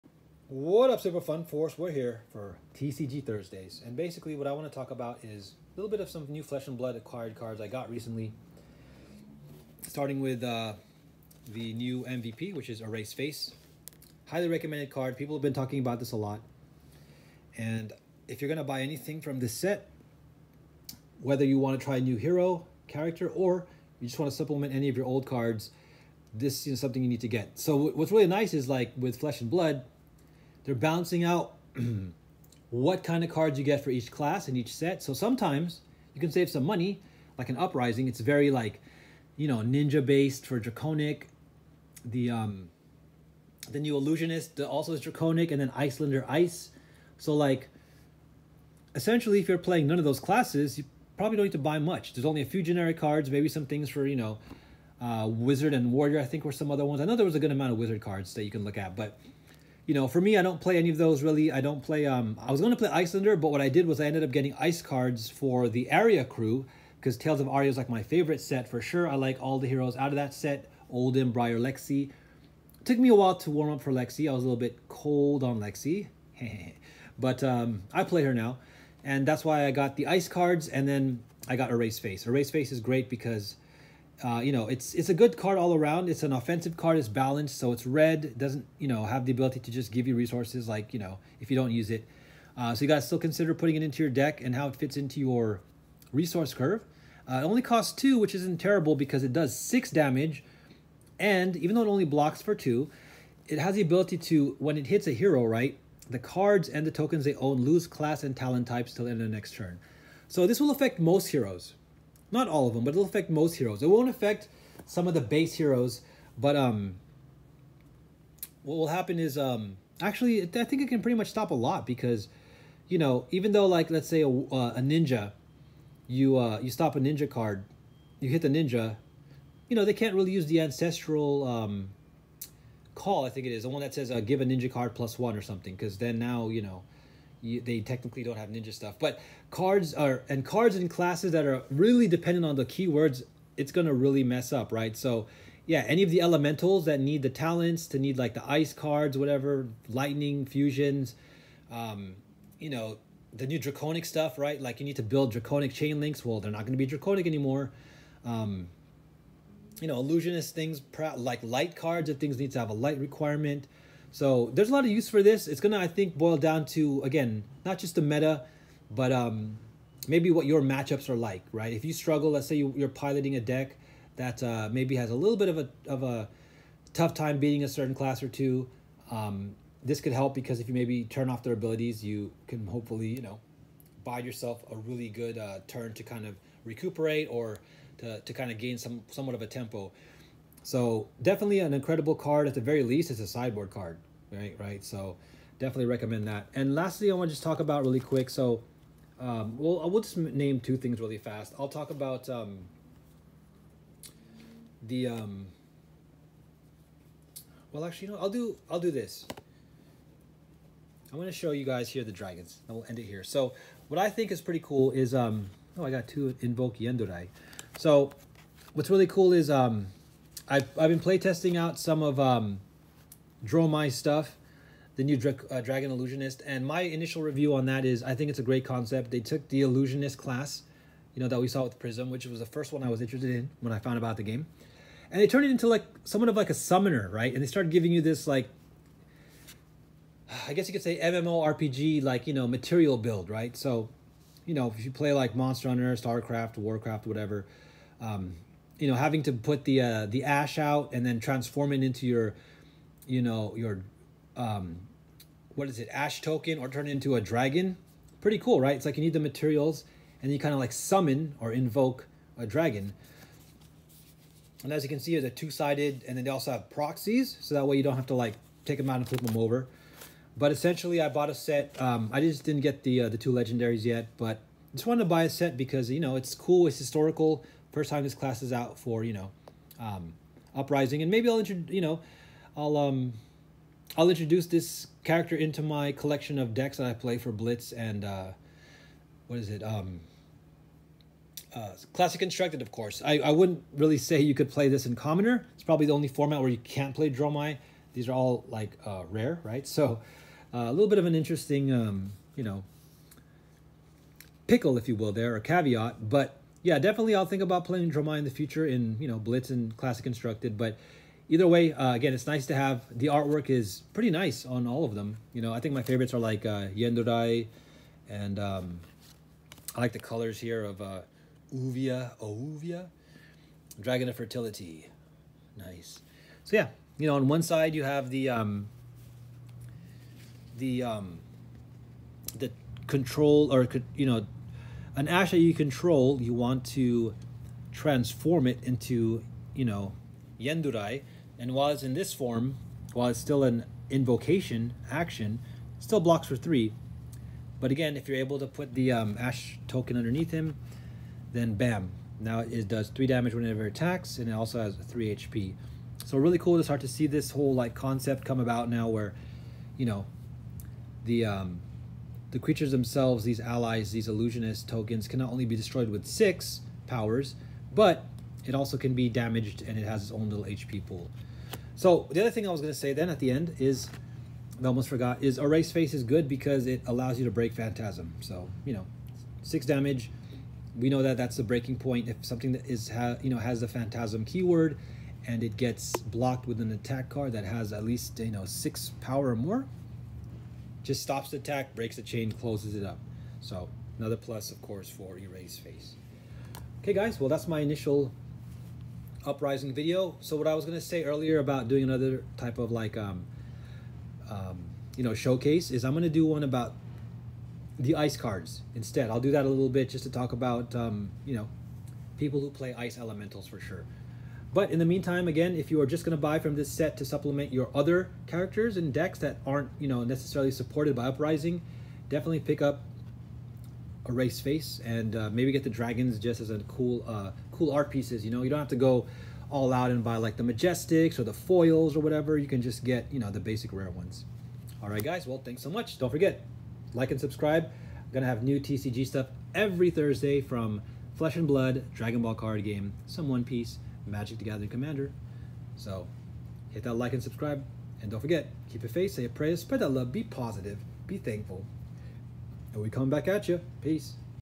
Flesh and Blood Uprising Tips
In this feature, I talk about a must-have card for most decks and some cards I picked up for my collection.